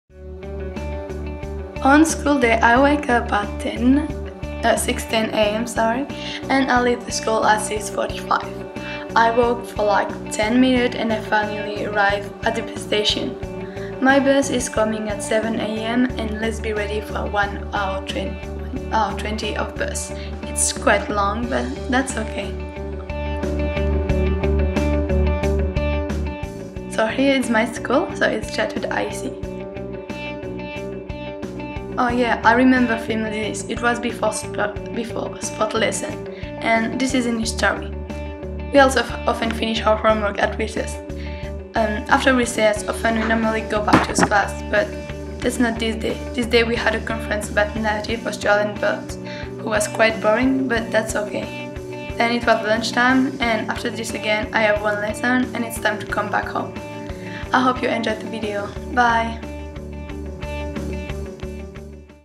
DAY IN MY LIFE: French in an Australian High School (CHATSWOOD IEC)
One day with me at my school in Sydney
I m going to a special school called CIEC (Chatswood Intensive English Center). This is a school for international students who come from all around the world
We have a lot of fun there and I m really happy to go!!

To all my CIEC's friends, I miss you all of you guys hahaha 😘